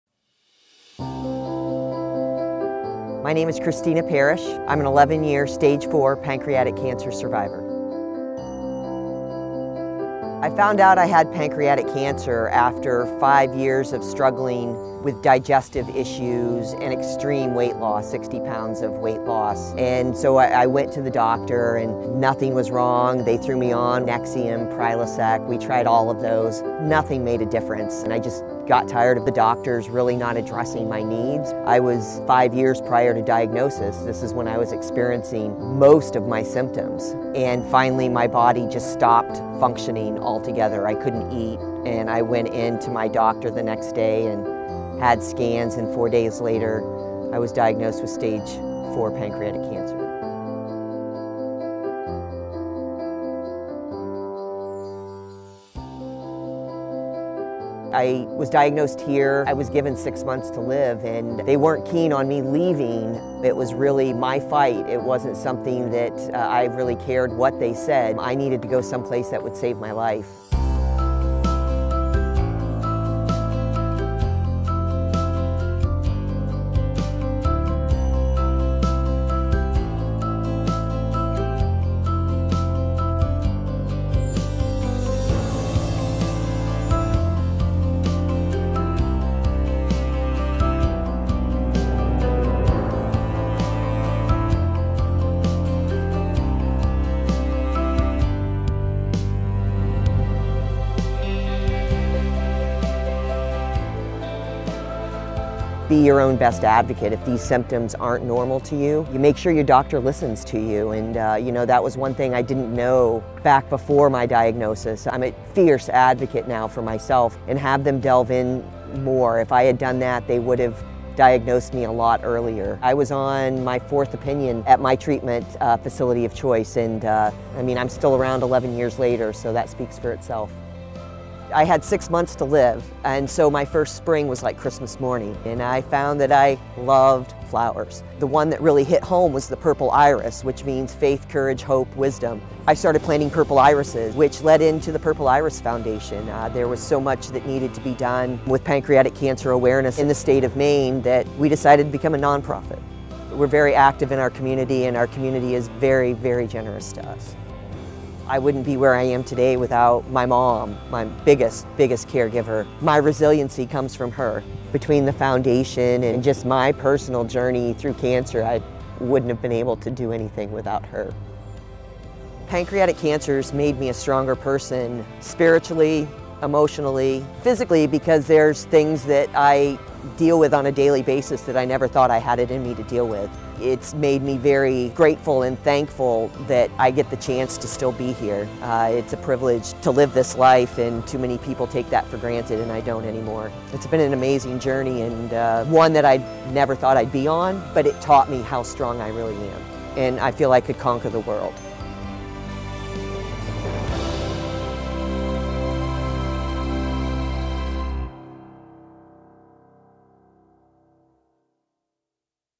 Chris Parrish -- Be Your Own Best Advocate
After years of digestive problems, Chris Parrish was diagnosed with stage IV pancreatic cancer. It was 2008, and she was 40 years old.

Rather than accept her prognosis, Chris searched for a place that she believed would save her life. She traveled with her mother to Cancer Treatment Centers of America in Chicago.  Throughout her treatment, her mother was her biggest source of support.

Chris founded the Purple Iris Foundation to give back and provide support for other people facing cancer.